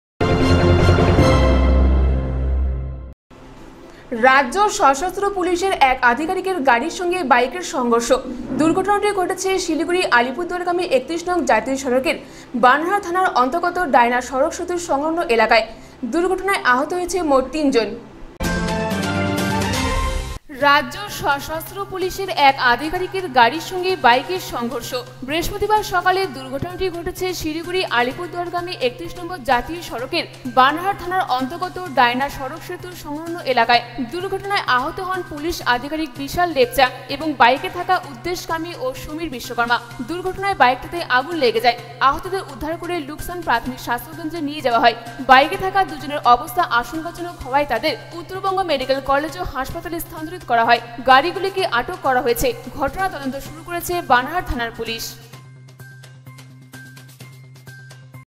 রাজ্য সশস্ত্র পুলিশের এক আধিকারিকের গাড়ির সঙ্গে বাইকের সংঘর্ষদুর্ঘটনাটি ঘটে শিলিগুড়ি আলিপুরদুয়ারগামী
রাজ্য সশস্ত্র পুলিশের এক আধিকারিকের গাড়ির সঙ্গে বাইকের সংঘর্ষদুর্ঘটনাটি ঘটে শিলিগুড়ি-আলিপুরদুয়ারগামী ৩১ নম্বর জাতীয় সড়কে